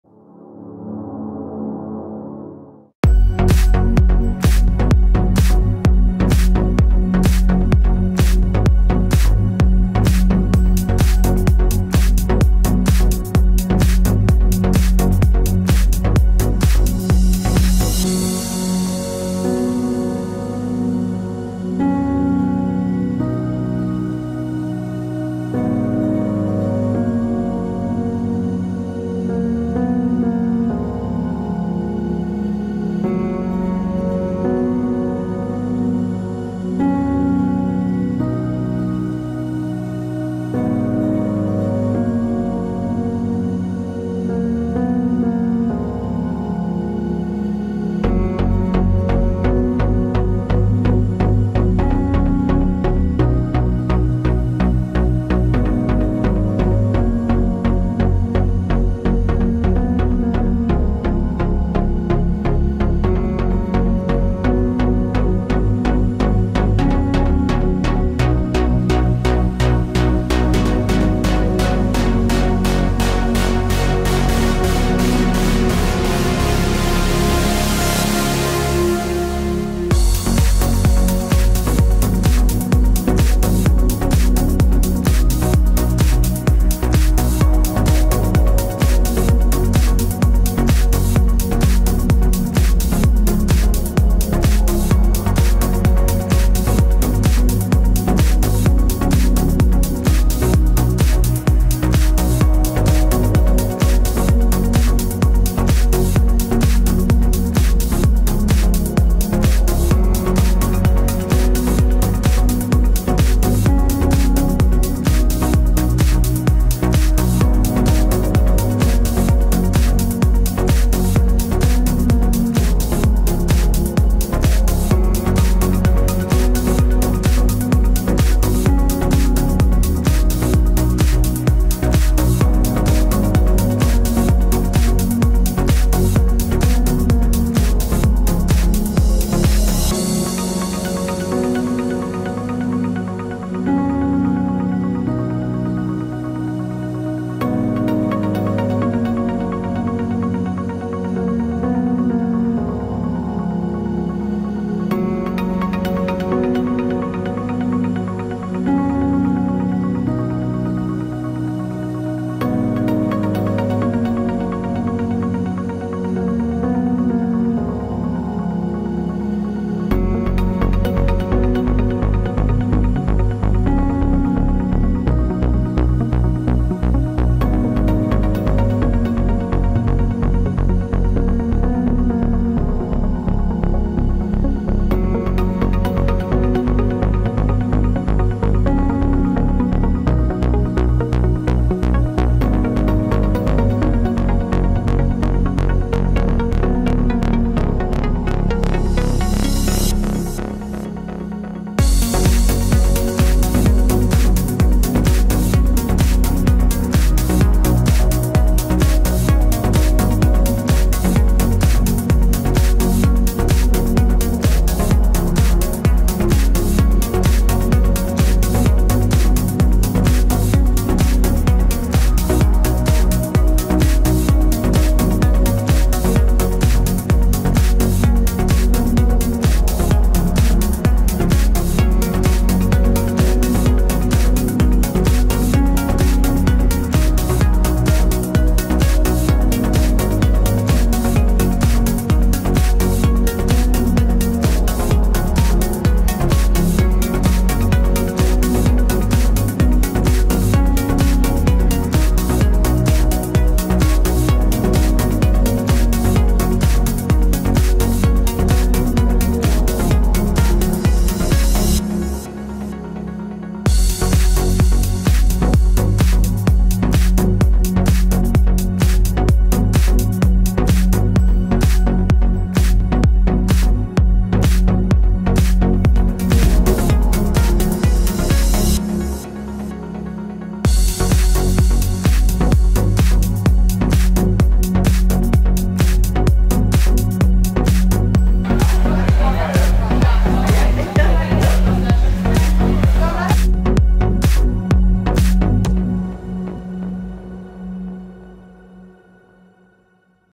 POMPEII ancient city in Italy
Pompeii  an ancient city in Italy,  buried by Vesuvius.Tour among the ruins. A short visit to Naples.